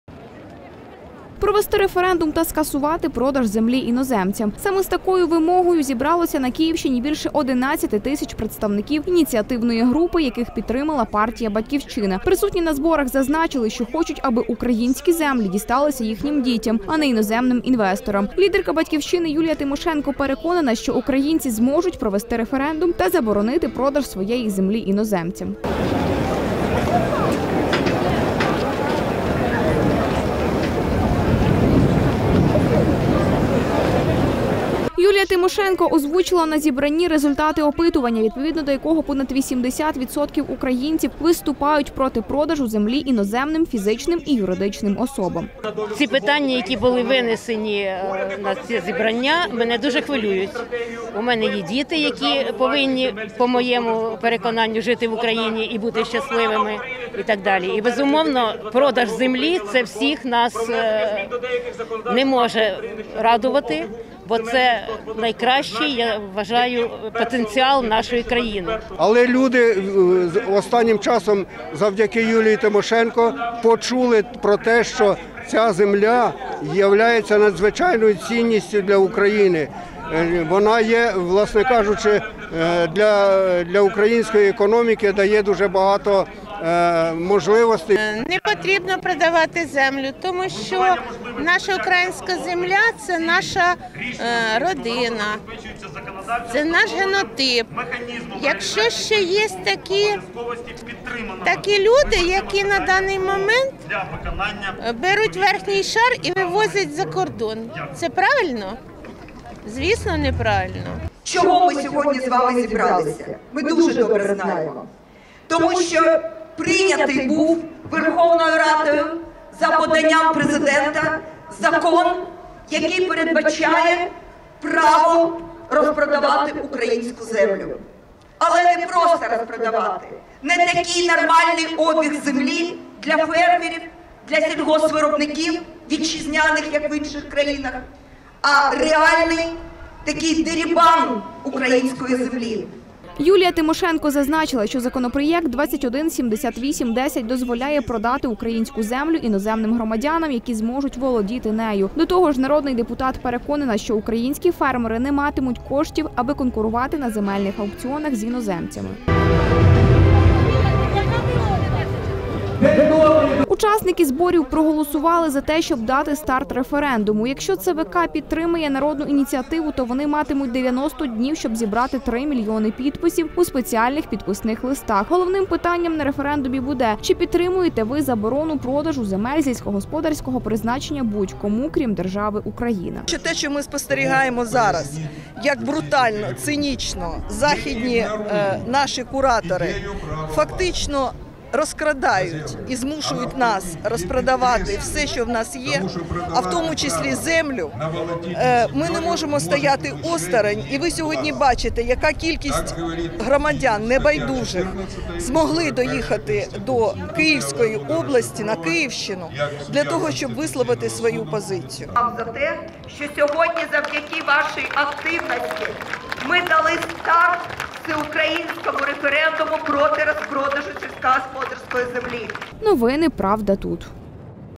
Старт до Всеукраїнського референдуму проти продажу землі покладено
Провести референдум та скасувати продаж землі іноземцям. Саме з такою вимогою зібралося більше 11 тисяч  представників ініціативної групи, які підтримала партія «Батьківщина». Присутні на зборах зазначили, що хочуть, аби українські землі дісталися їхнім дітям, а не іноземним інвесторам. Лідерка «Батьківщини» Юлія Тимошенко переконана, що українці зможуть провести референдум та заборонити продаж українських земель українцям.